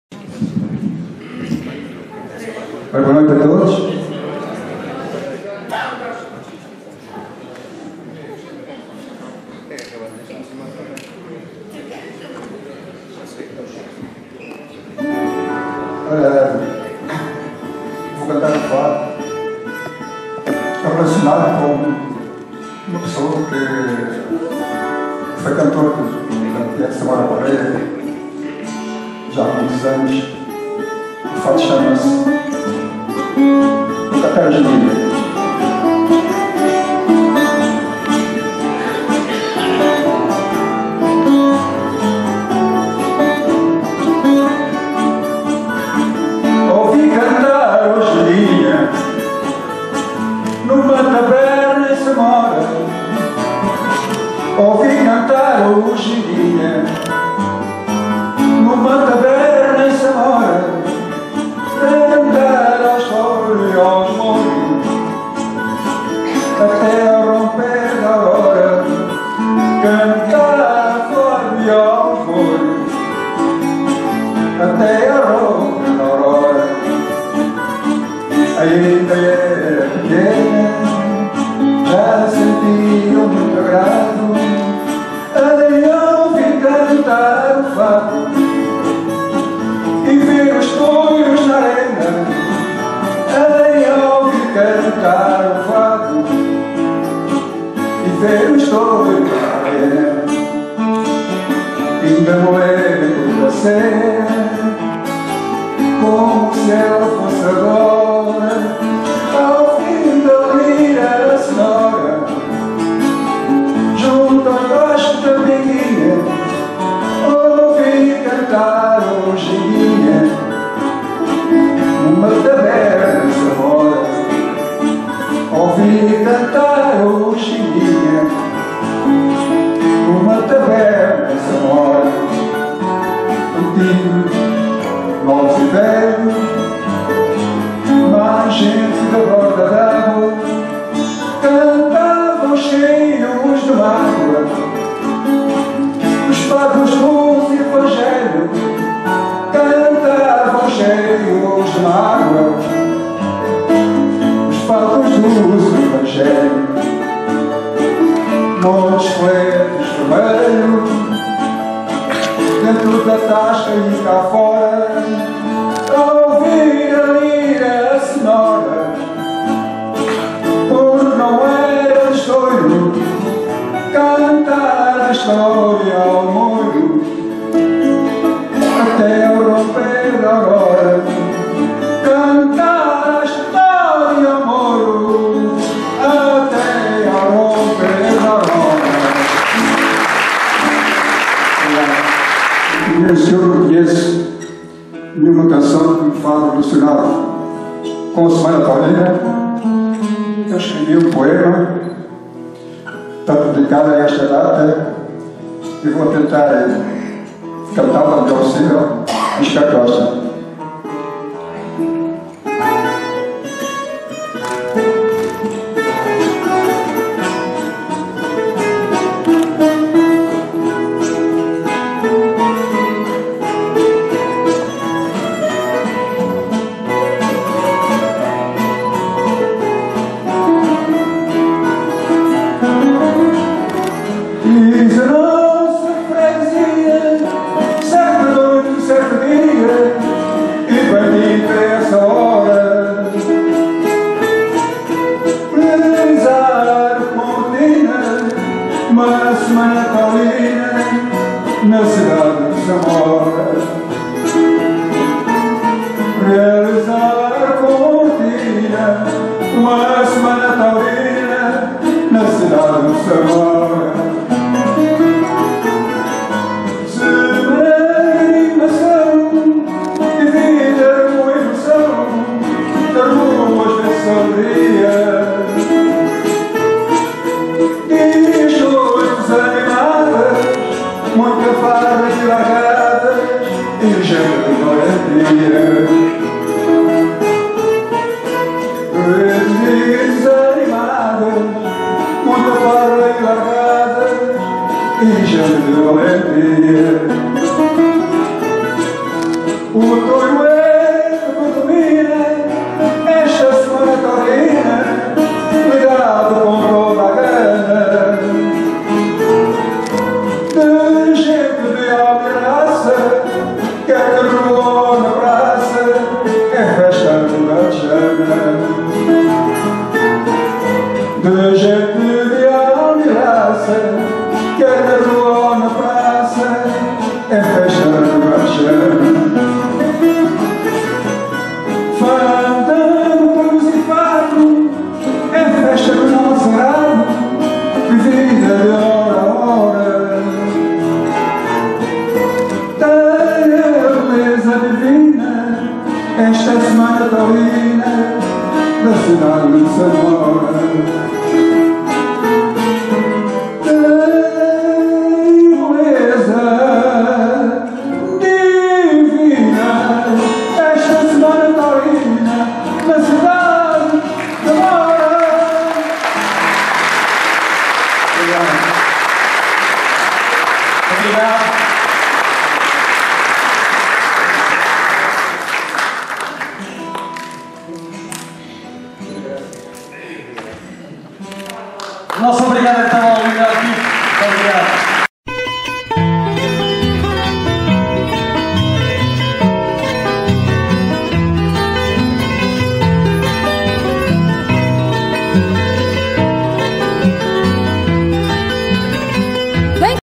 2022 05 05 - FADOS E GUITARRADAS - LEONEL TITO
Espetáculo incluído na XV Semana Taurina Samora Correia.